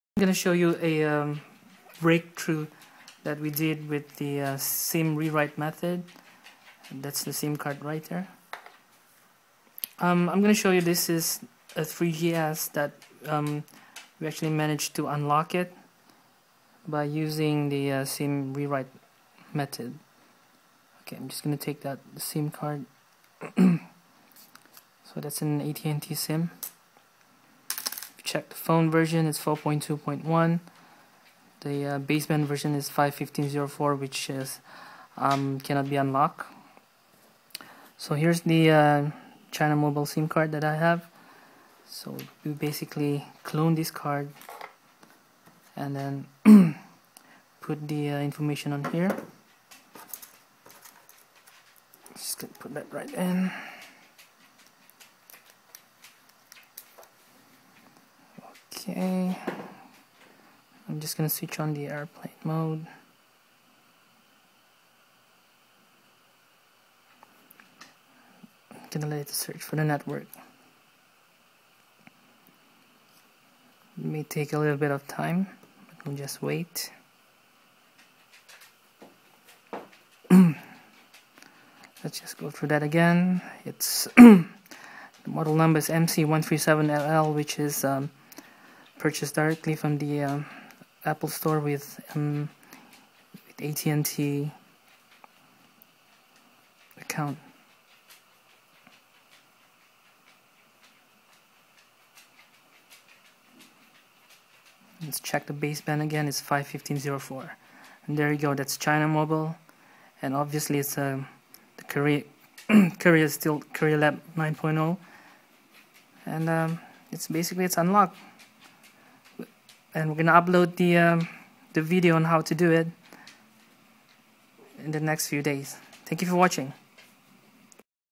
Unlock iPhone 3G, 3GS Baseband 5.14.02, 5.15.04, 5.16.02 with SIM Re-write Method
iPhone 3G, 3GS with baseband 5.14.02, 5.15.04 and 5.16.02 can now be unlocked using SIM card re-write method.

This video shows an iPhone 3GS, model number MC137LL purchased from Apple Store with AT&T contract, unlocked with a China Mobile SIM card.

If you don't wish to update to baseband 6.15.00 to unlock your iPhone, this is your ultimate and safest solution. It will not affect your GPS!

This also works on iPhone 4 on all baseband versions. No jailbreak is required.